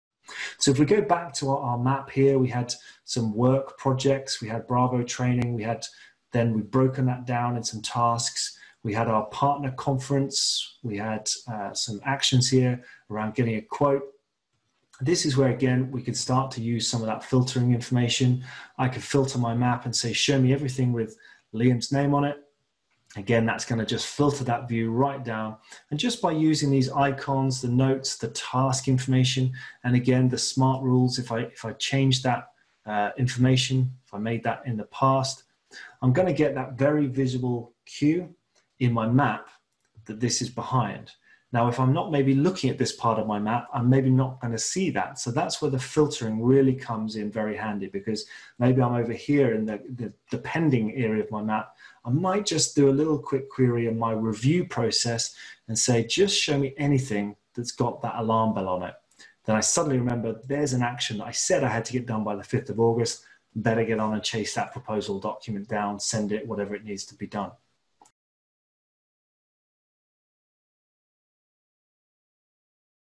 Getting Things Done with MindManager (PREVIEW)
In this PRO webinar, we explore the ways @MindManager can be used to support you in implementing the David Allen "Getting Things Done (GTD)" approach for managing your personal organisation and productivity! View full recording